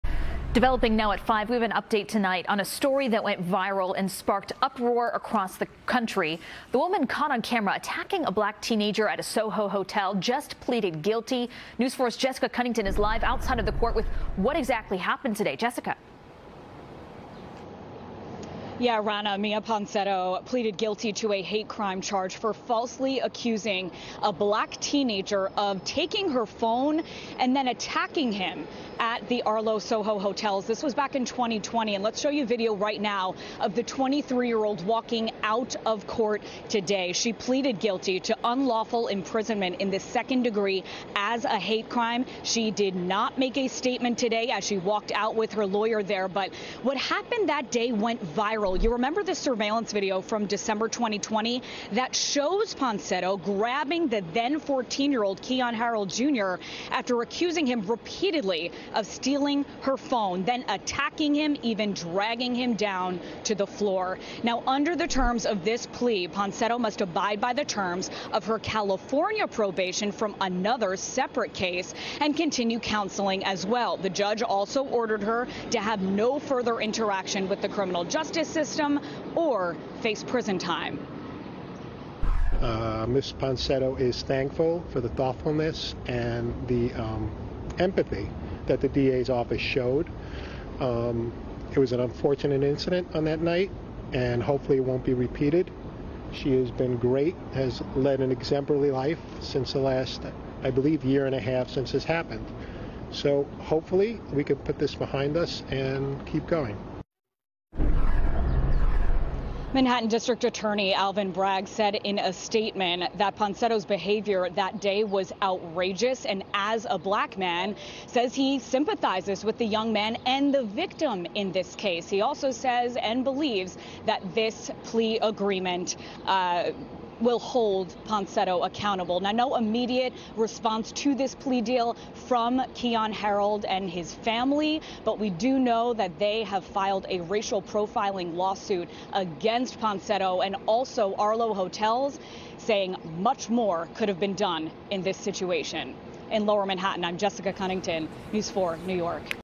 Miya Ponsetto Pleads Guilty in Caught-on-Camera Attack on Keyon Harrold Jr. | NBC New York
The California woman charged with a hate crime for allegedly wrongly accusing a Black teen of taking her phone at a Manhattan hotel in December 2020 and grabbing at him as he tried to leave has pleaded guilty, the district attorney's office said Monday.

Miya Ponsetto had previously pleaded not guilty last summer to charges including unlawful imprisonment as a hate crime, aggravated harassment and child endangerment in the fray that unfolded at the Arlo Hotel with then-14-year-old Keyon Harrold Jr.